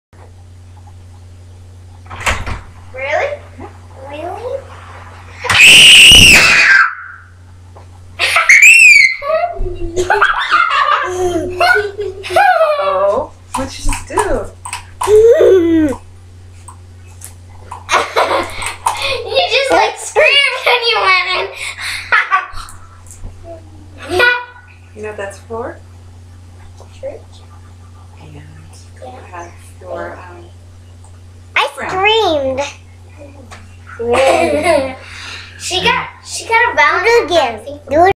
Briaunna's surprise Valentines gift
Briaunna entering in her bedroom to find her Valentine dress waiting for her.